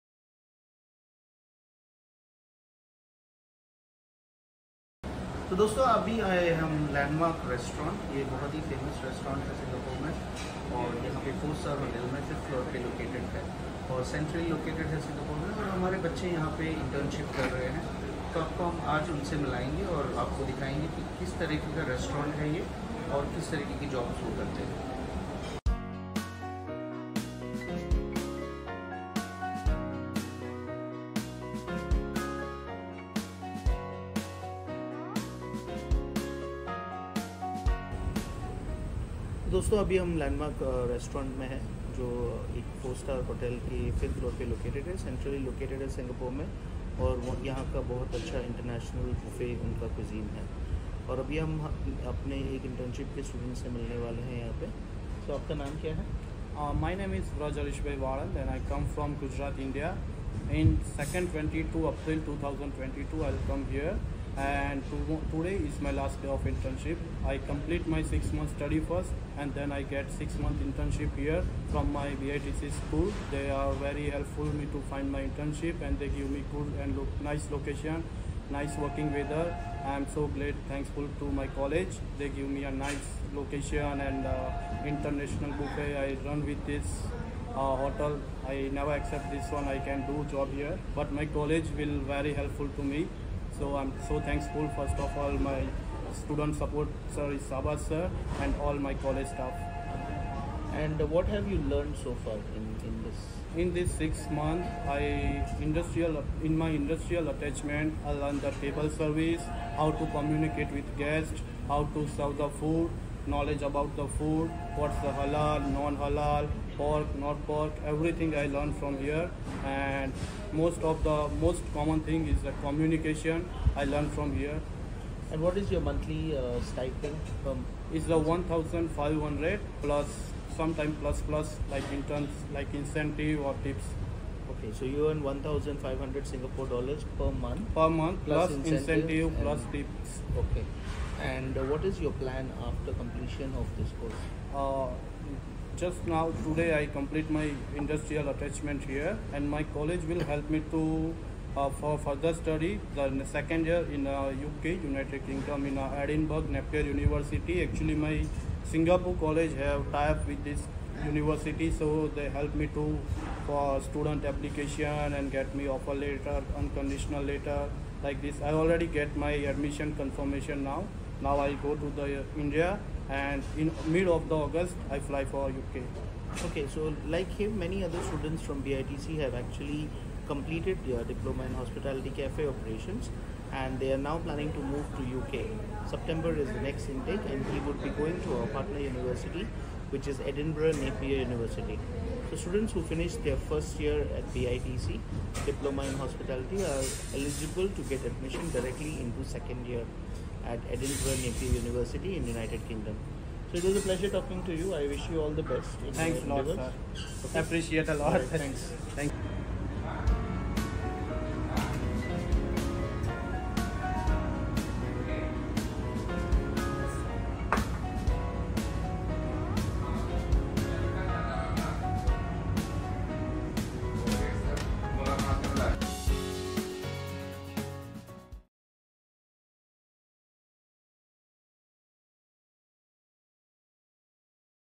I A Partner Visit - LandMark ( English）
Industrial Attachment is a compulsory and integral part of the diploma course. Students at BITC will be deployed to related industries Industry for a period of 6 months. What is their real working environment like? What did they learn from the internship? How much salary do they get each month? Follow BITC's Senior Manager SHADAAB SHAIKH, visit BITC IA Partner - Landmark Hotel, visit and interview students on the spot.